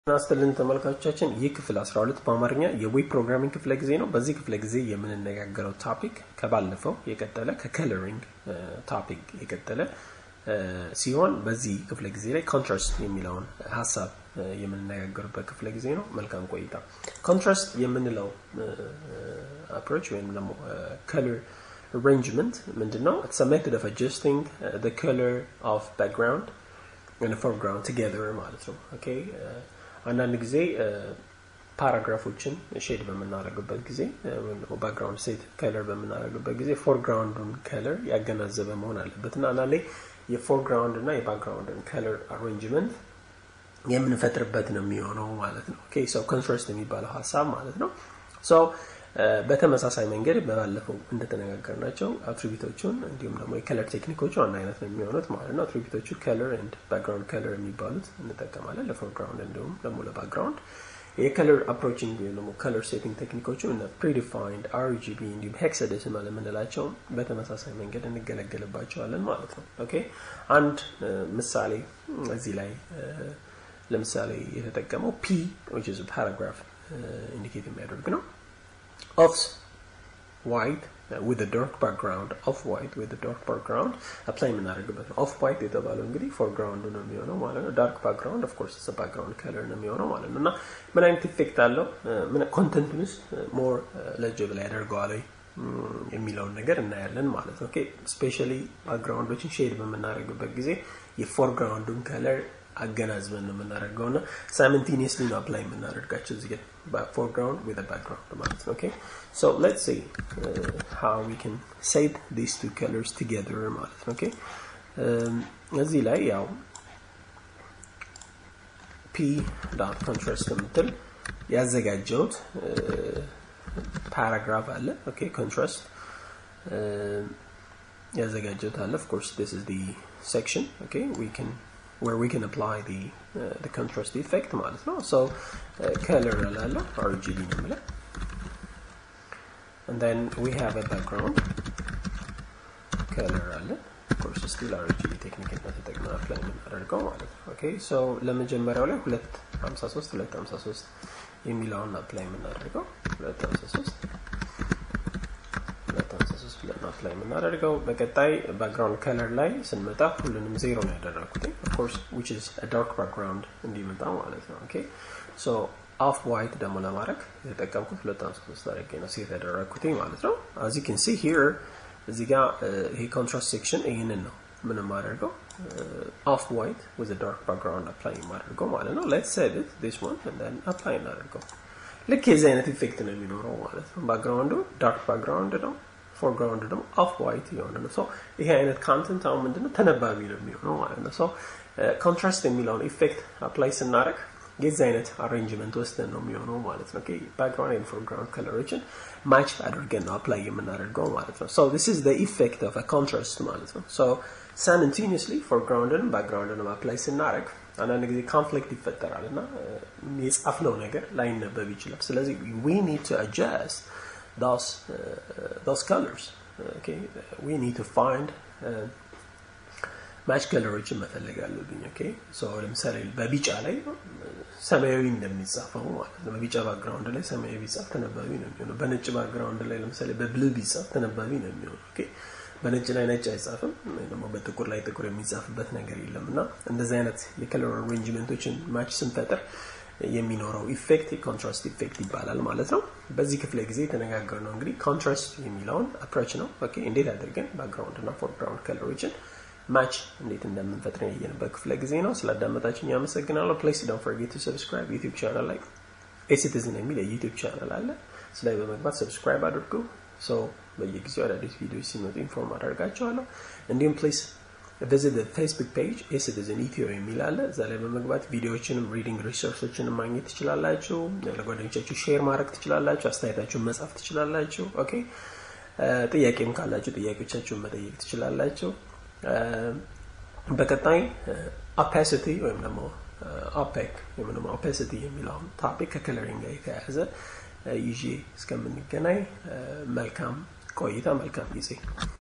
CSS Coloring Contrast
This video teaches you the technique how a color contrast effect is applied for the content of the web page. The technique combines the attributes color and background-color. Further reading resource is available in the following Dropbox link